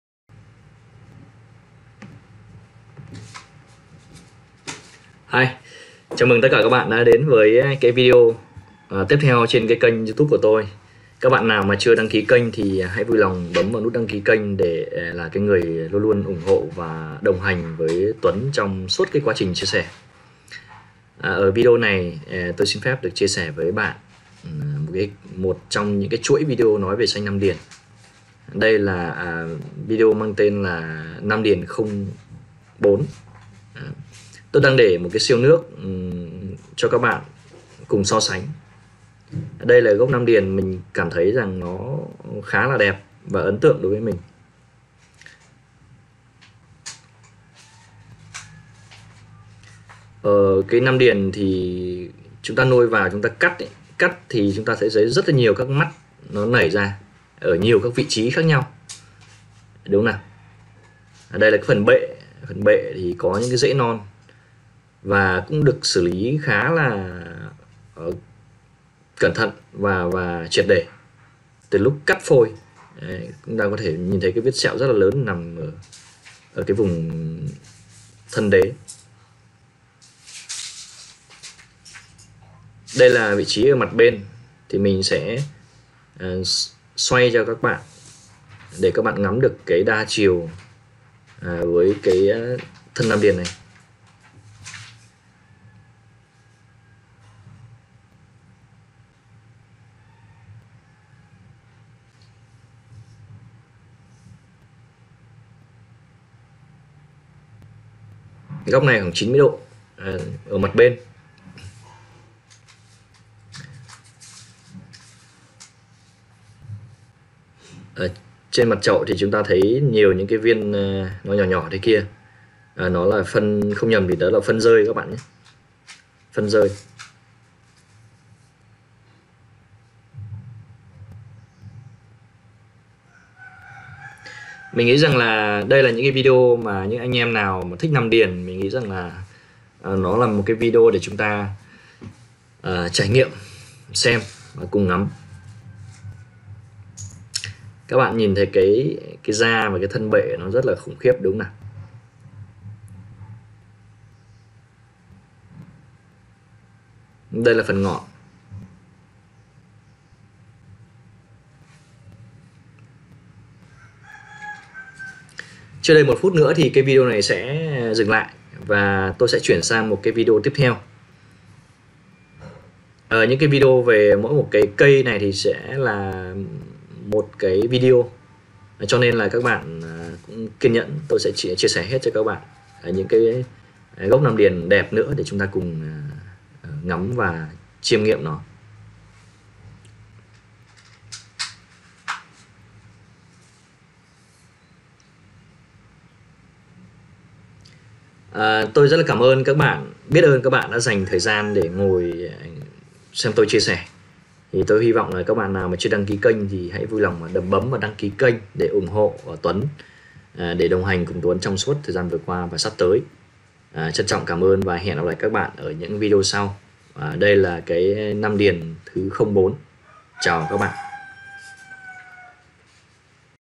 Nghĩ cho kỹ trước khi mua SANH NAM ĐIỀN, đừng ân hận bản thân khi chọn sanh rác
NGOÀI RA BẠN CÓ THEO DÕI

CHÂN TRỌNG CẢM ƠN!

  Mến chào và hẹn gặp lại vào ngày mai!